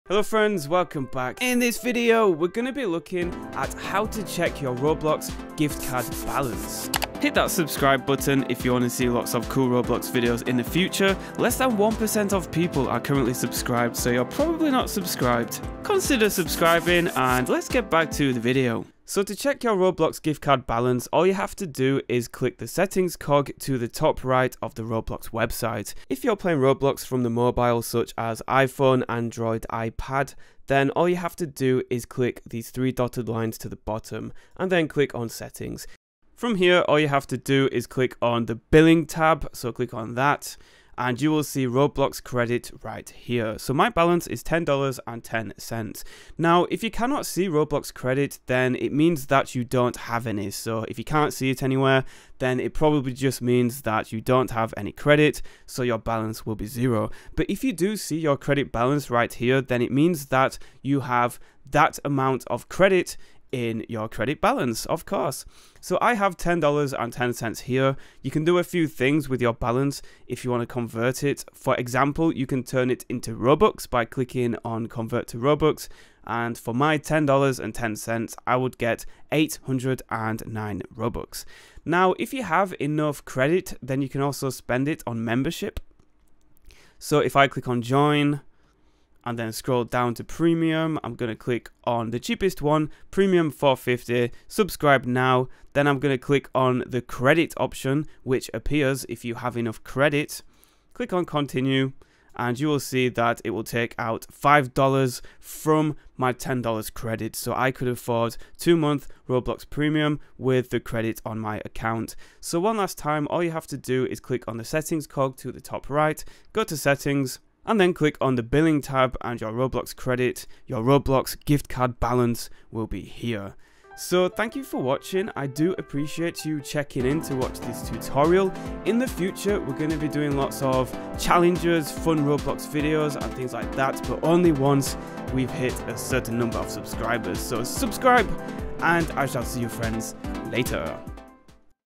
How To Check Your Roblox Credit Balance 2021 - How To Check Roblox Gift Card Balance (PC Mobile)
How to check your roblox gift card balance On Roblox 2021. this quick and easy tutorial will show you how to check your roblox Credit balance! :D

To check your roblox credit balance on the PC, mobile, iPad, android, Ios and more, simply head over to the settings as shown in the video. We will be looking at how to check your roblox gift card balance on both the mobile and PC. I hope this video helped on "How To Check Your Roblox Credit Balance"